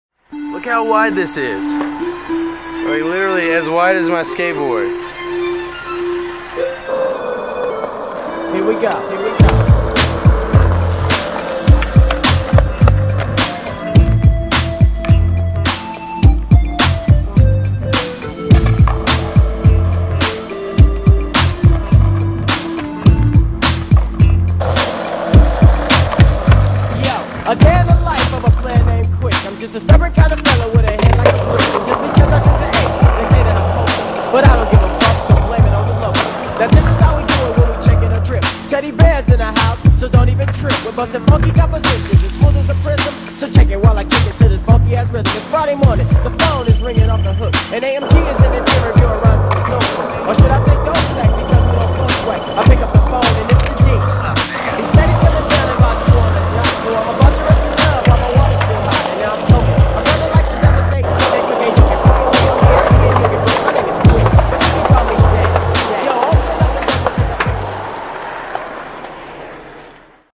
Tuesday Ten Getdown #5 Middletown Wal-Mart
Missletown Walmart
Filmed By: Giles Scialdone
Chance White Skateboarding